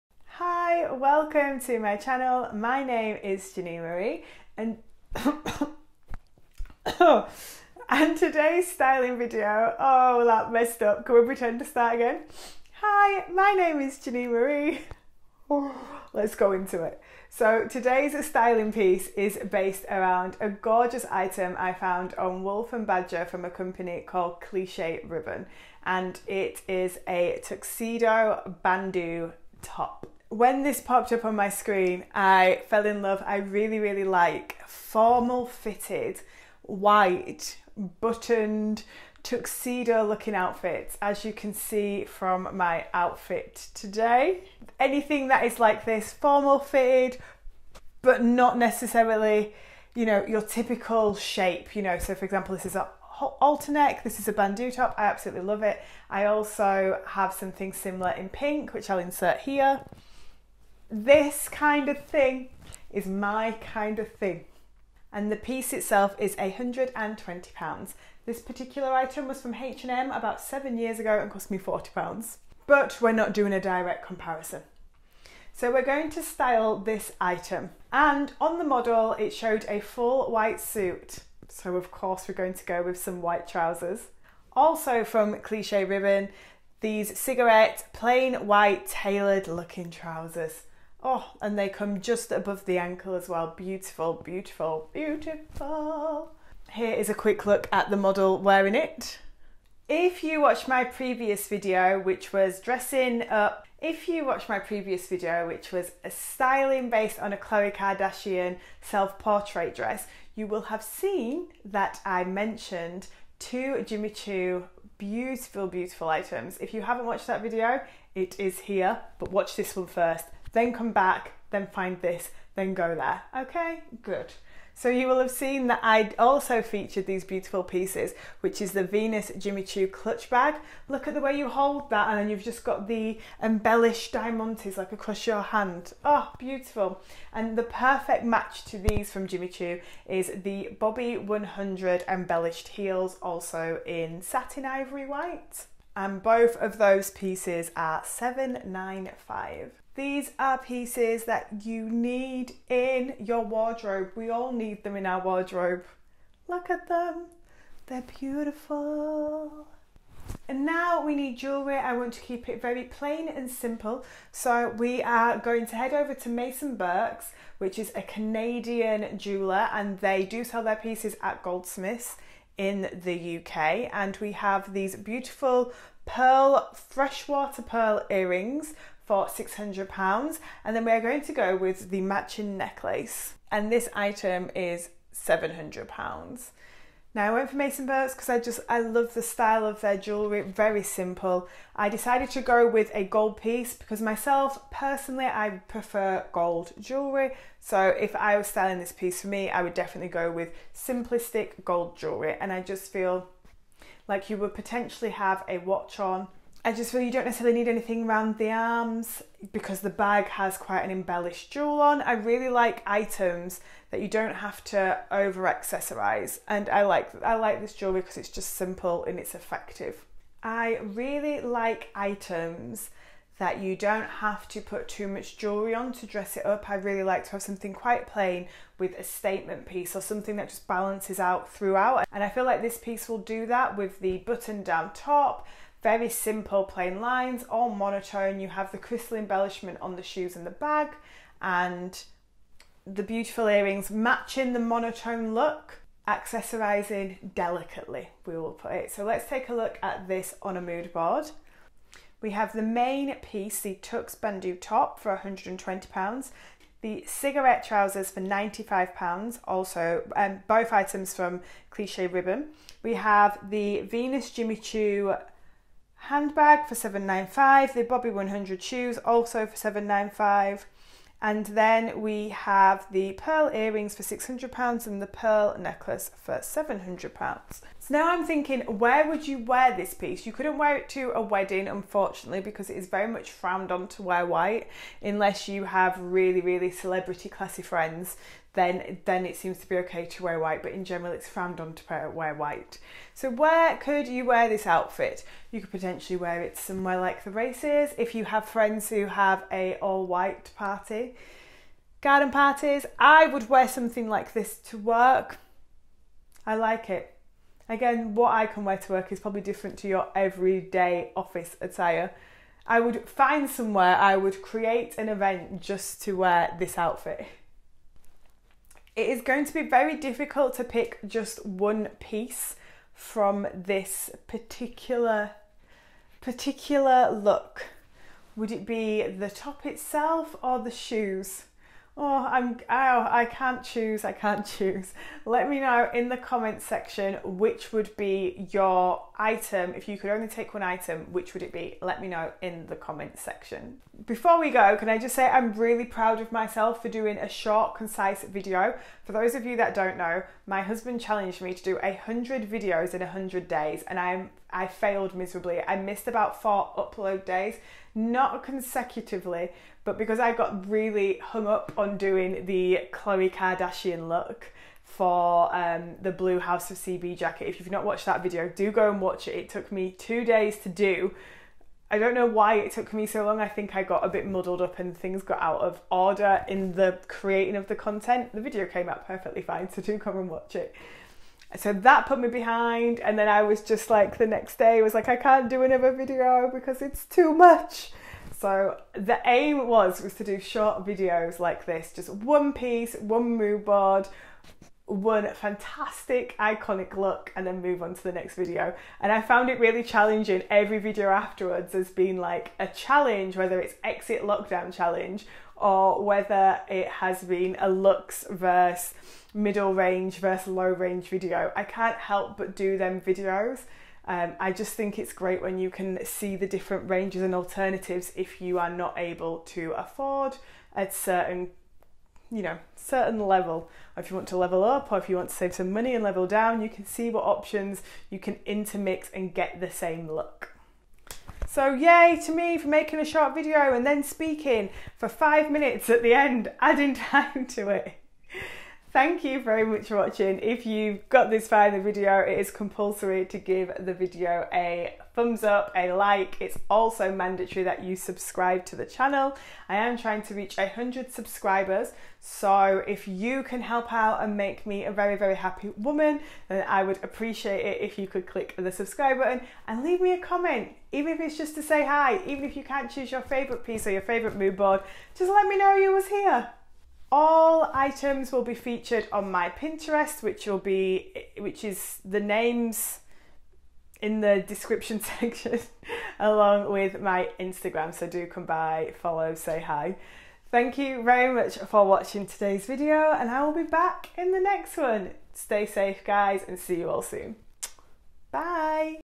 What to wear with a white Suit | Style | Janine Marie
Today's items are from Wolf & Badger, Jimmy Choo, and Maison Birks